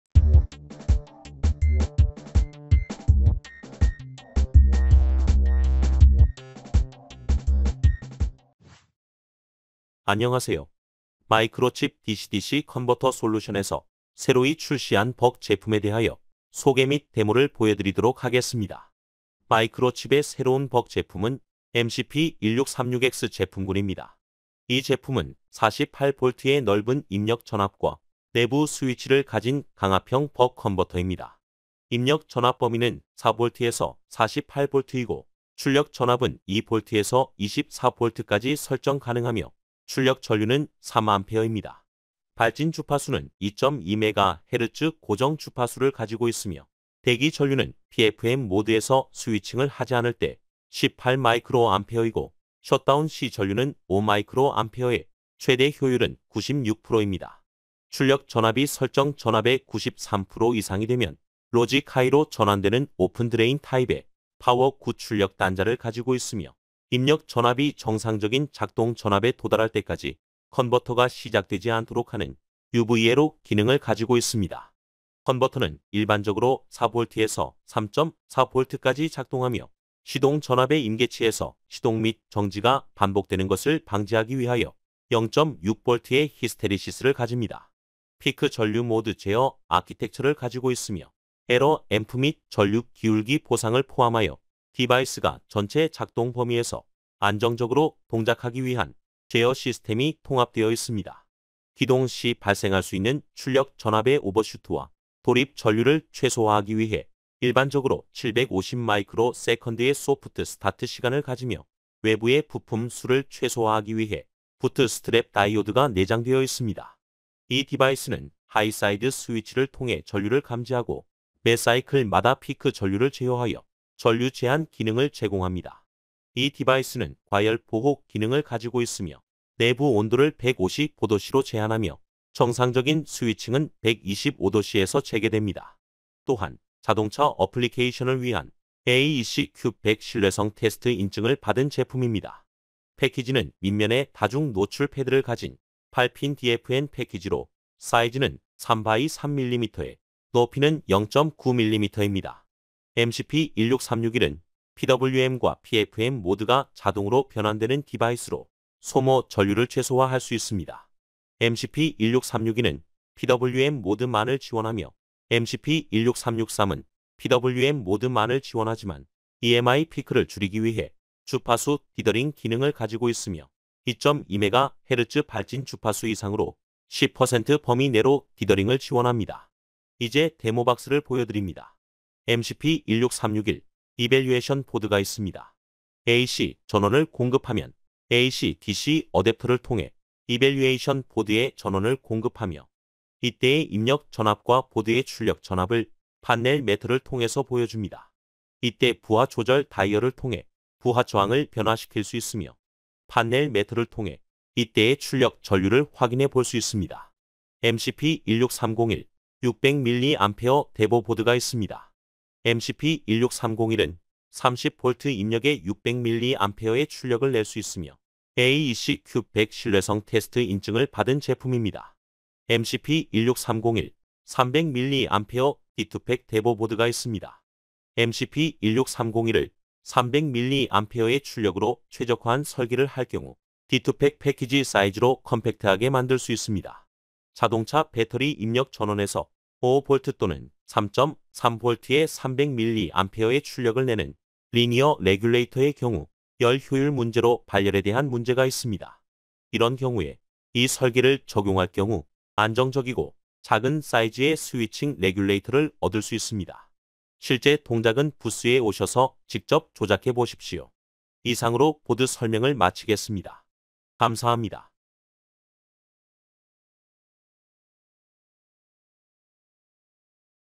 48V의 넓은 정격 입력을 가진 자동차용 강압형(벅) 컨버터
이 영상에서는 마이크로칩이 새로이 출시한 48V의 넓은 정격 입력 전압을 가지며, 내부 스위치를 가진 강압형 컨버터에 대한 소개 및 데모를 보여드리도록 하겠습니다.
MCP1636x 제품군은 4V ~ 48V의 입력 전압에 2V ~ 24V까지 출력 전압을 설정할 수 있으며, 3A 전류 출력이 가능합니다.
2.2MHz 고정 발진 주파수를 가지고 있으며, MCP16363은 Frequency Dithering 기능을 가지고 있어서 EMI의 Peak를 줄일 수 있으며, 3x3x0.9mm 8pin DFN 패키지를 가지고 있습니다.